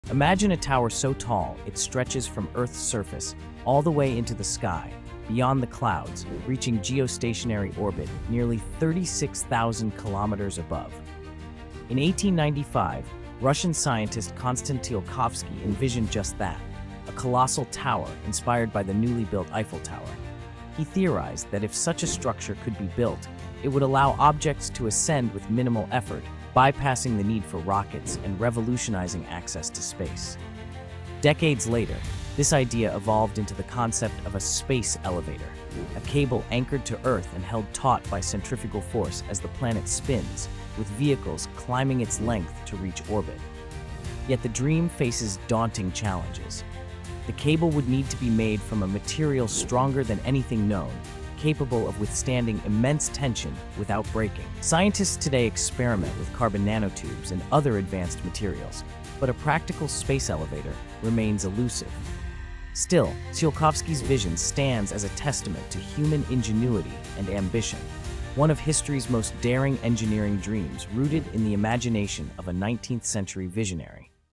The Space Elevator: A Dream from 1895
Did you know the idea for a space elevator dates back to the 19th century? Discover its incredible history and the science behind this mind-bending concept!